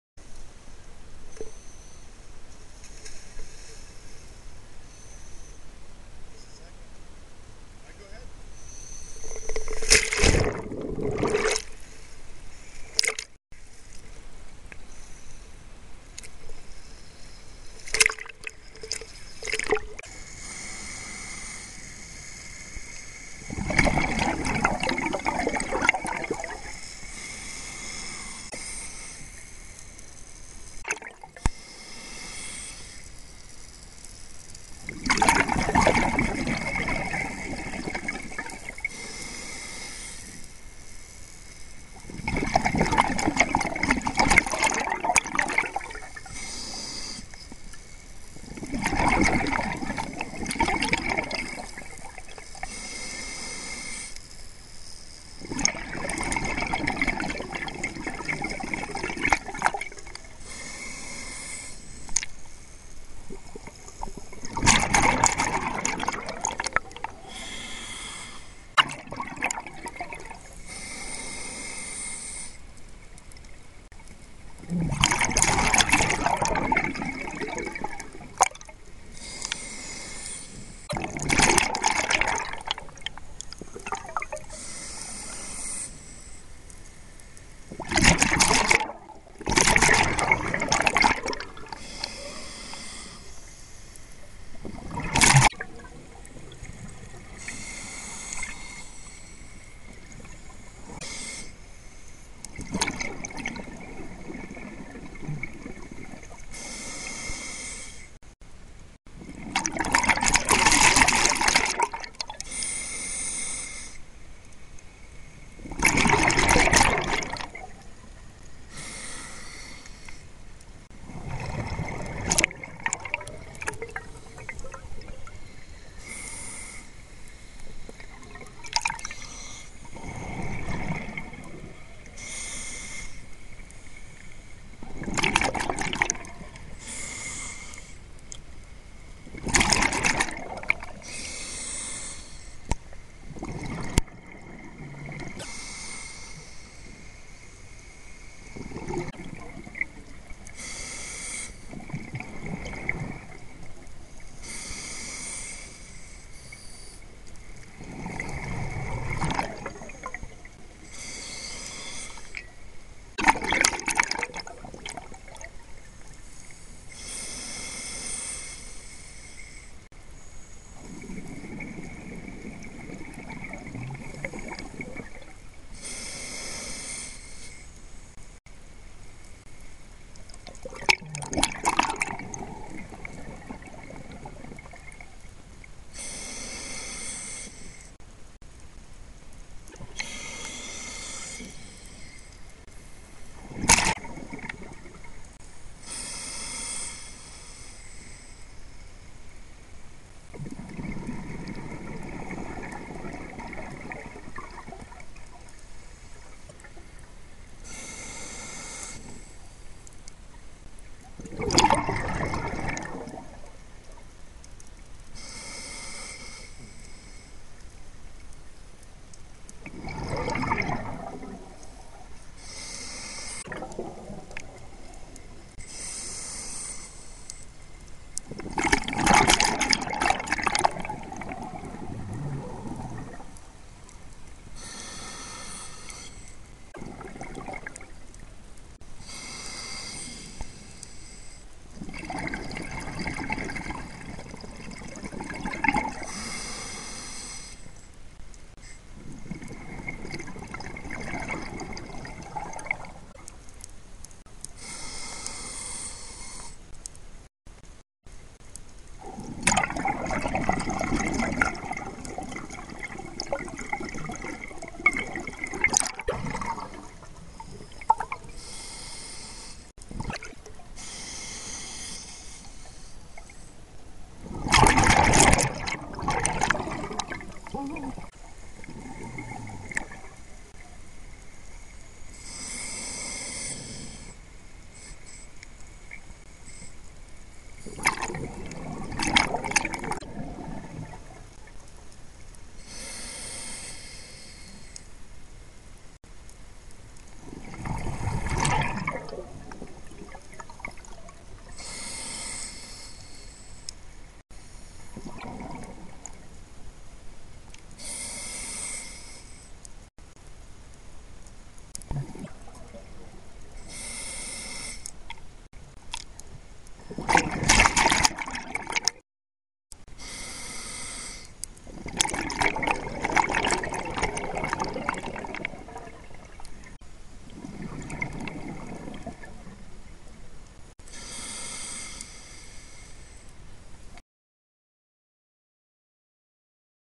United Caribbean Wreck
Diving on the wreck of the United Caribbean. Using an Easy Shot Clip to capture the video. Due to computer problems, a free editing software was used. Just enjoy it for what it is as the best I can do at the moment.